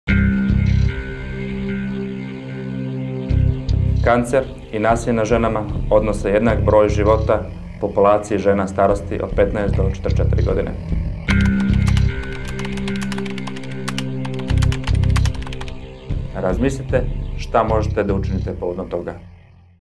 Kancer i nasilje nad ženama
14 činjenica o rodno zasnovanom nasilju.